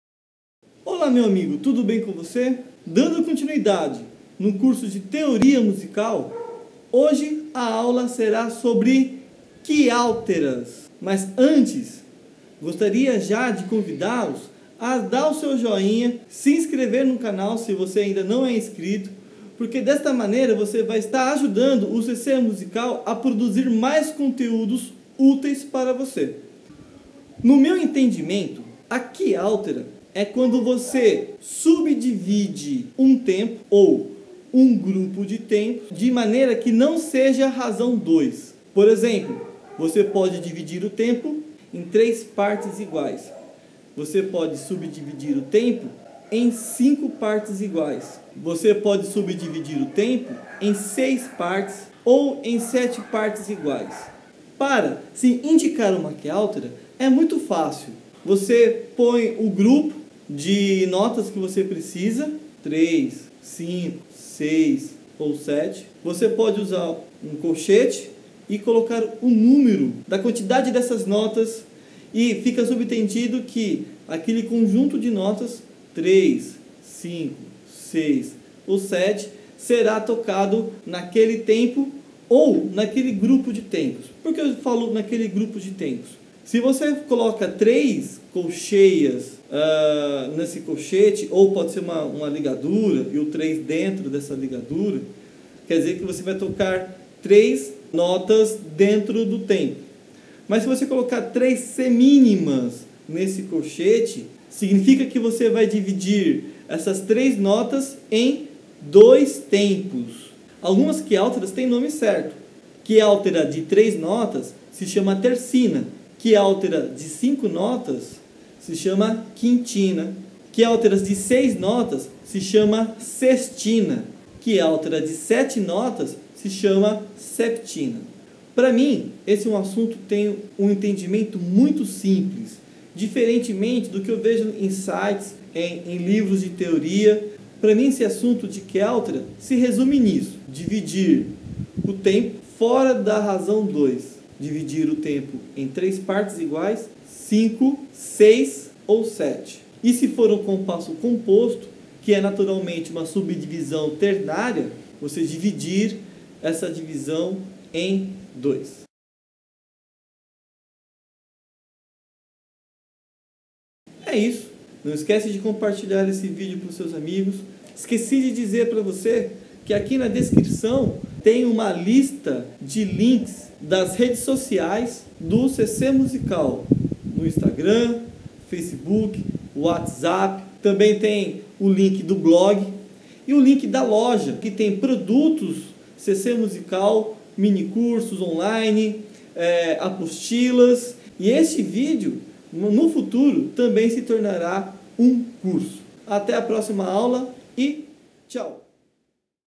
Aprender TERCINAS e outras QUIÁLTERAS | Teoria Musical
Olá! QUIÁLTERAS e todos os seus tipos como TERCINAS, QUINTINAS, SEXTINAS entro outras mais são os temas abordados neste vídeo de maneira fácil, direto e sem "teorismo". É minha compreensão do assunto, que vc não verá em lugar nenhum.

Tudo é uma questão de dividir 1 tempo em vários pedaços diferentes.  É só assistir!

Bora tocar?
Bom estudo.